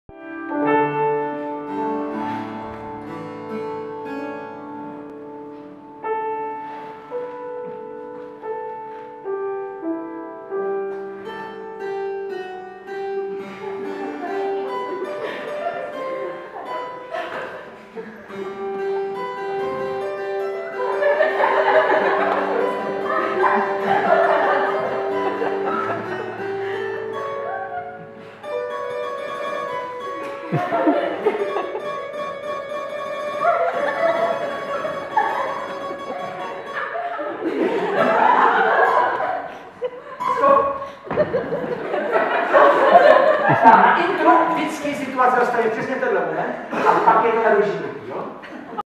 Divadlo Terapie - Ars Terapeutica III.
Divadlo Terapie pod vedením Mgr. Josefa Zajíčka na Mezinárodní konferenci uměleckých terapií "Ars Terapeutica III., která se konala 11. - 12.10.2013 v Olomouci ve spolupráci s VOŠ sociální CARITAS Olomouc, Akademií Alternativa s.r.o. a MAUT - Mezinárodní asociací uměleckých terapií. Více